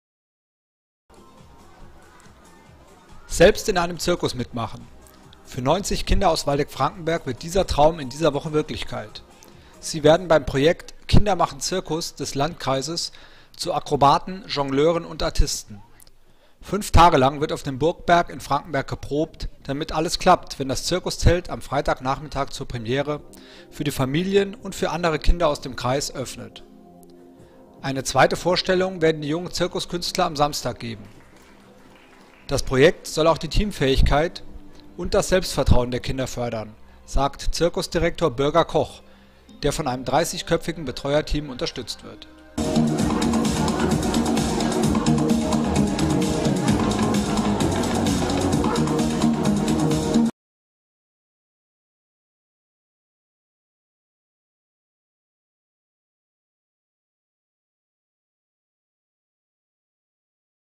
Zirkus-Projekt in Frankenberg für Kinder und Jugendliche
Frankenberg, 25. Juni 2013: Der Landkreis Waldeck-Frankenberg bietet wieder Kindern die Möglichkeit, selbst Zirkus zu machen: Gerade haben die rund 90 Kinder und Jugendlichen, die am Projekt „Kinder machen Zirkus" teilnehmen, ihre einstudierte Nummer erstmals im Zirkuszelt vorgeführt und einen ersten Eindruck davon bekommen, was es heißt, im Rampenlicht zu stehen.